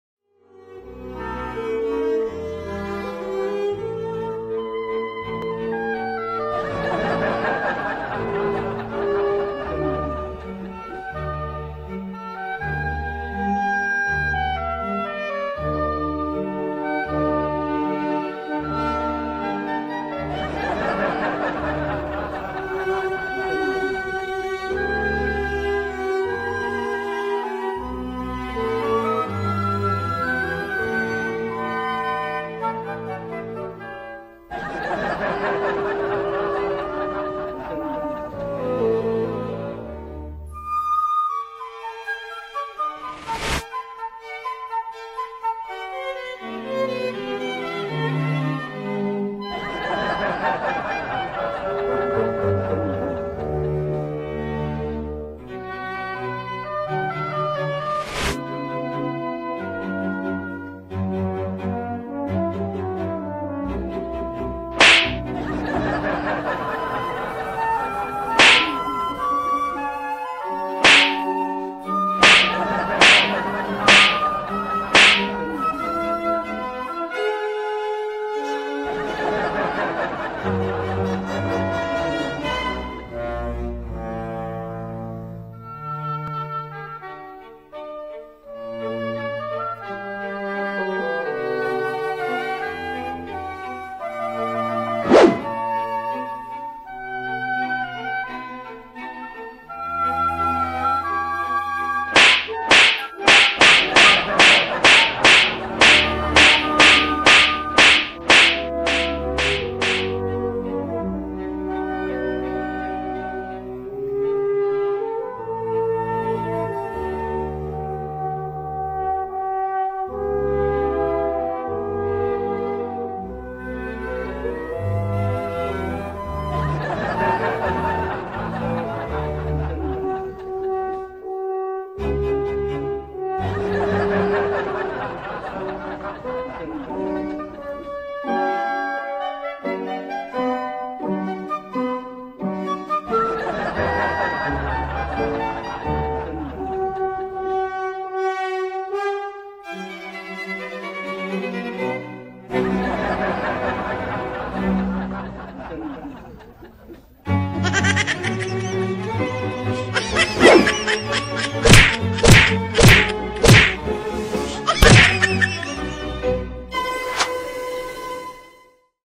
Don't trouble your mum EP1
you can't trouble your mum and  not get a share of the dangerous motherly weapon . wooden spoon and slippers .